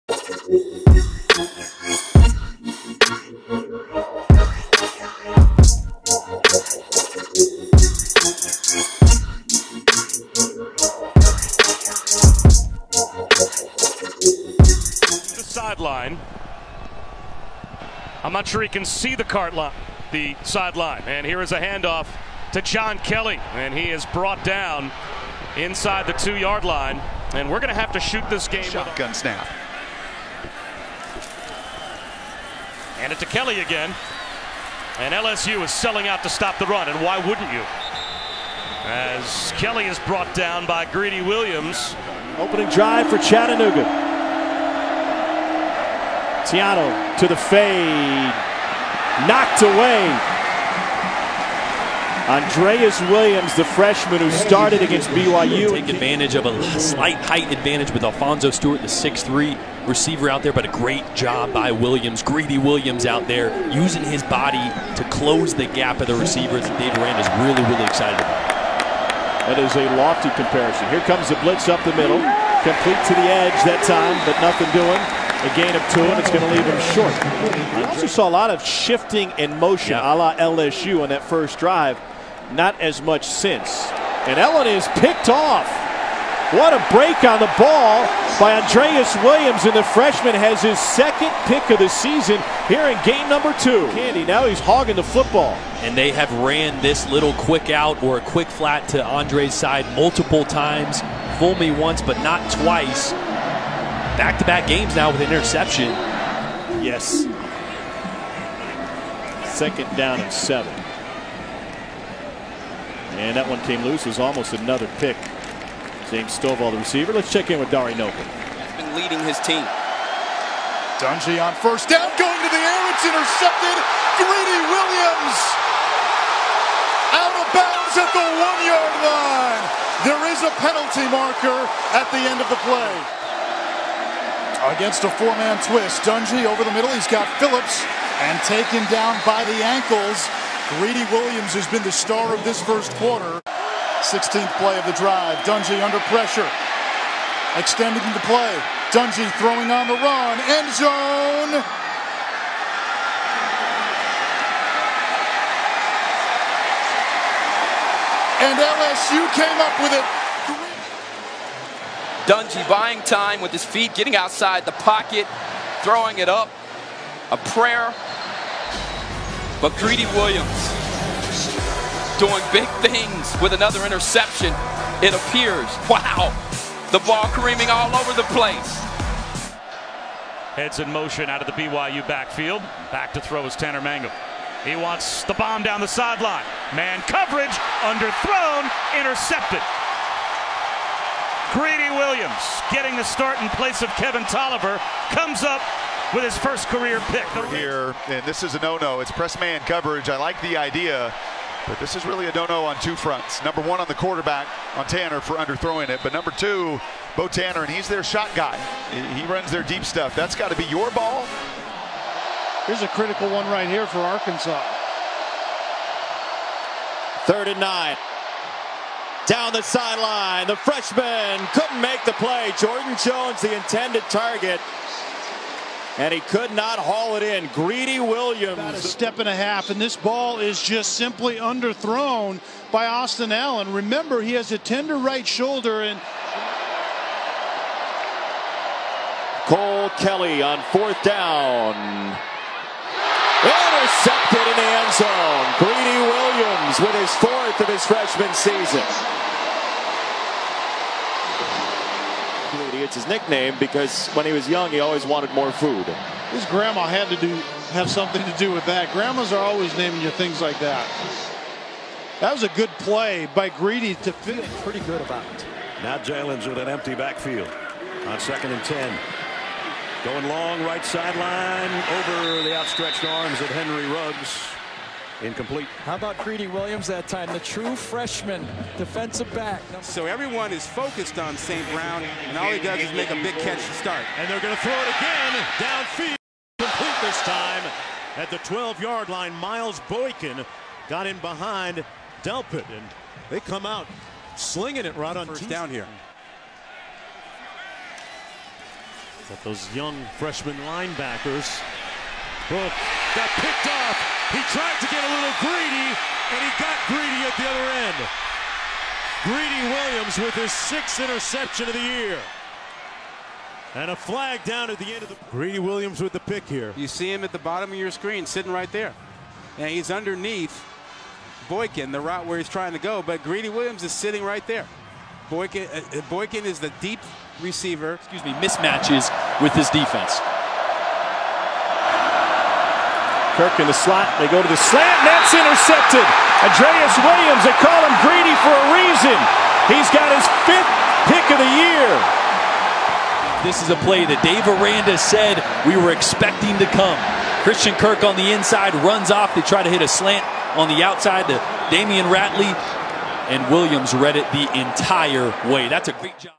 Andraez 'Greedy' Williams LSU 2017 Freshman Highlights ᴴᴰ
Highlights from LSU DB Andraez 'Greedy' Williams 2017 season.